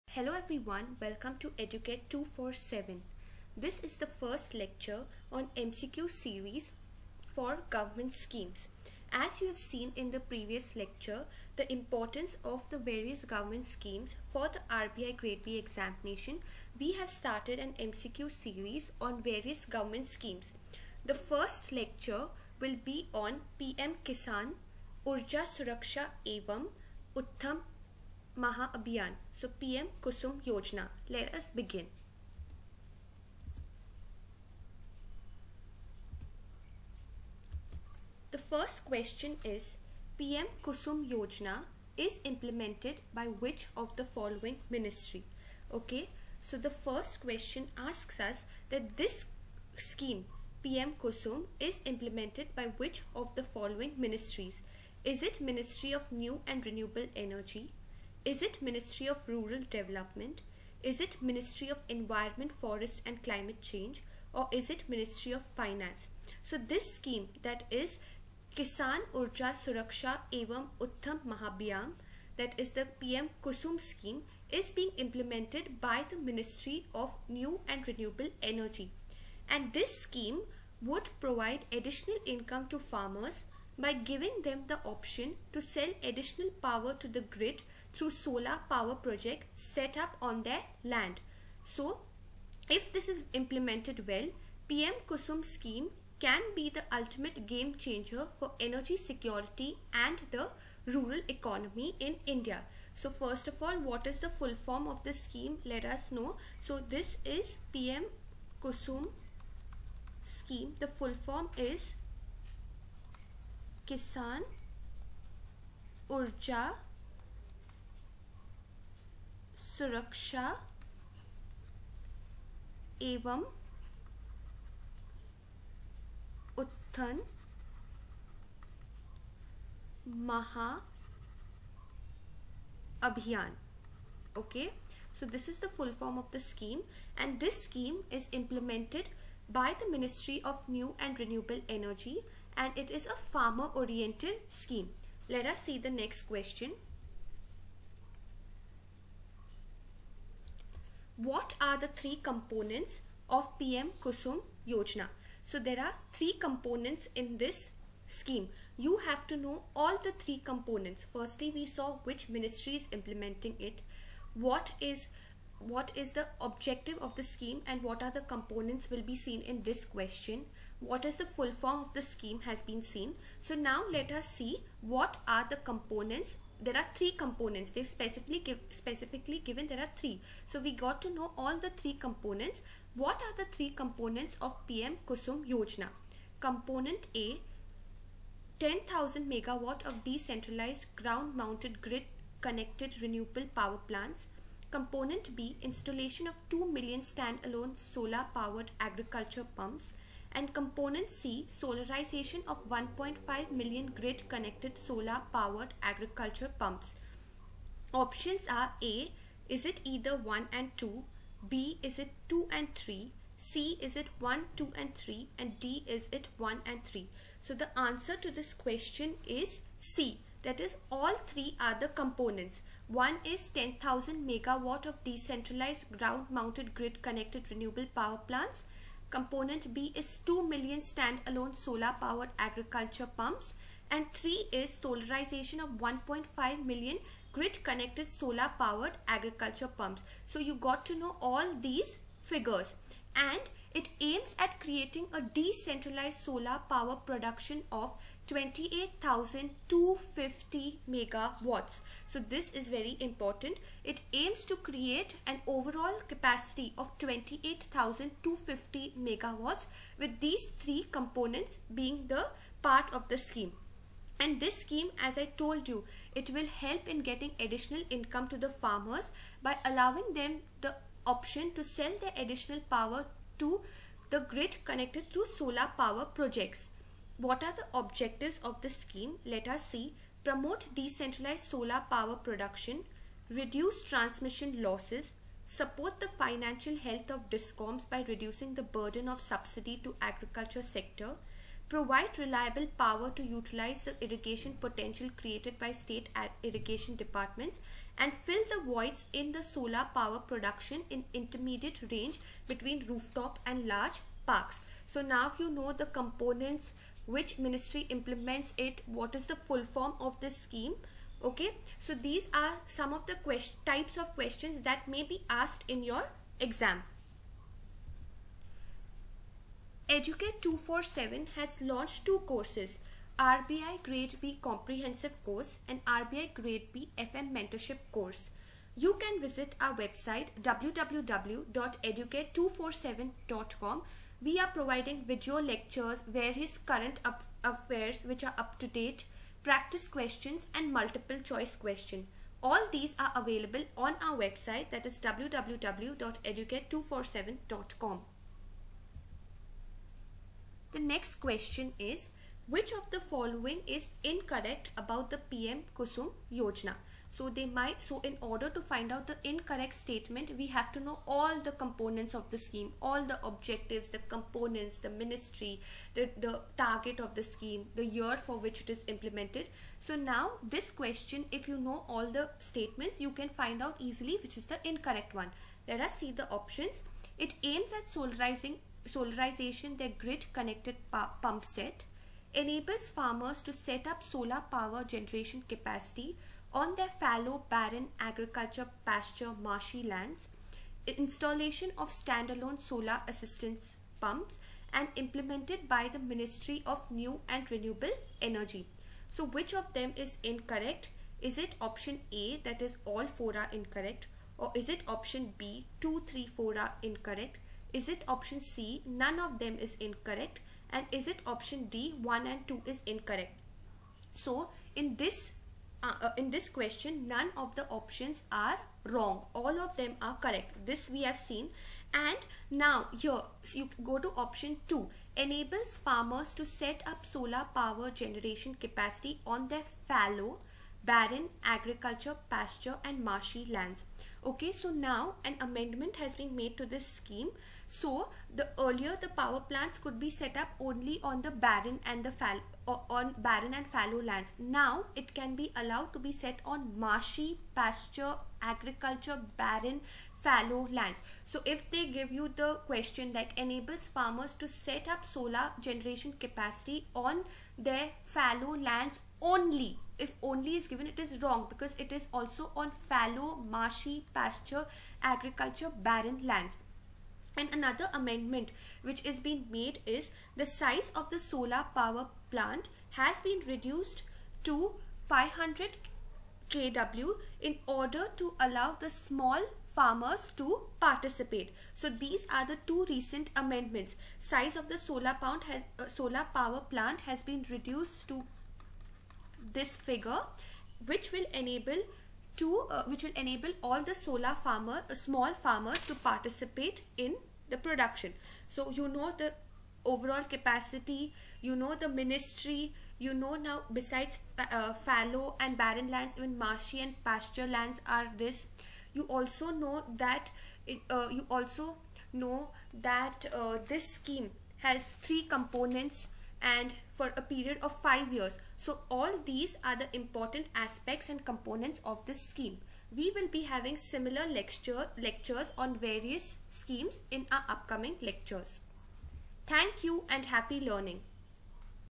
MCQs on PM-KUSUM | Government Schemes: Lecture 1 | RBI Grade B 2021
This video discusses the PM-KUSUM Scheme in the form of MCQs. It will be helpful to cover the current affairs for RBI Grade B.